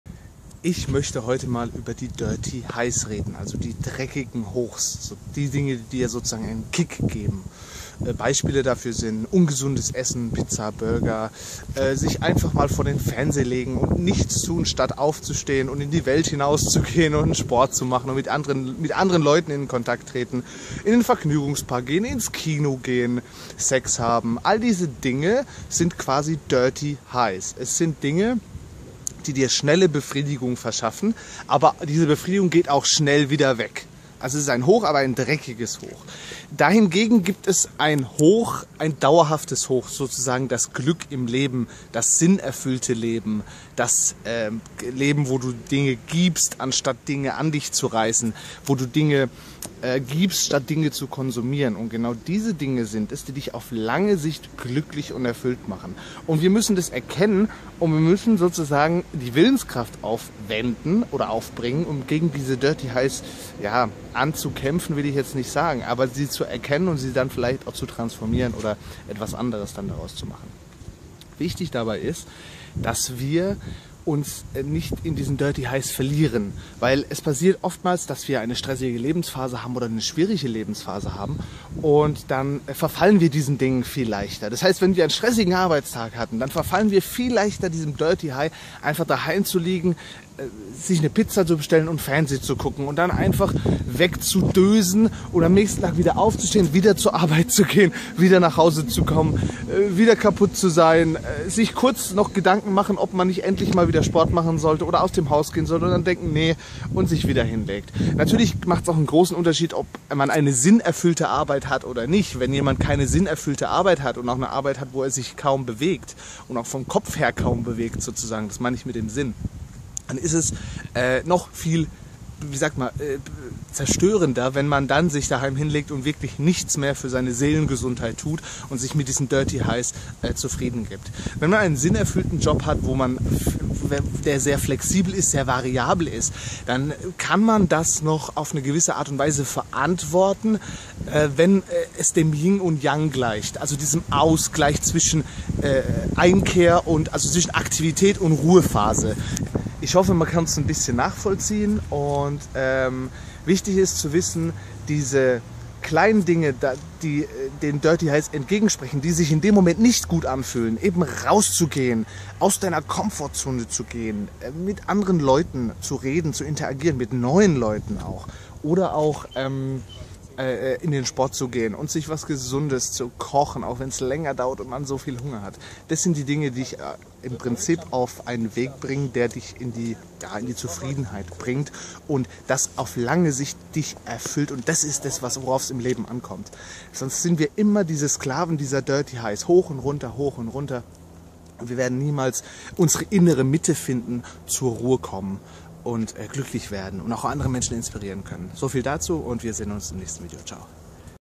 Dirty Highs (Dreckige Hochs) - kurzfristige Befriedigungen
Wie gut fühlt es sich doch an, nach der Arbeit einfach auf der Couch zu liegen, die Lieblingsserie zu schauen, sich eine Pizza zu bestellen und danach entspannt einzudösen.
Einfach mit Freunden ins Kino gehen, sich berieseln lassen, danach noch schön bei McDonalds vorbei und am Ende noch halbherziger Sex mit der Freundin um den "Druck" loszuwerden.

Das alles sind Dirty Highs !
Natürlich sind es Dinge, die in Maßen genossen, für ein ausgeglichenes Leben nicht schädlich sind. Jedoch ist unsere Gesellschaft so auf diese Dinge fixiert, dass diese längst ungesund und enorm desktuktiv geworden sind.